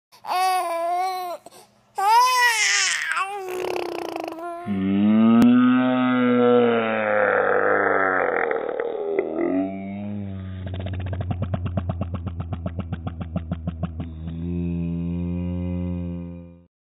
Baby Bruce Blubbering (Slow Motion)
He went through a phase of "blubbering" for a few months.  😊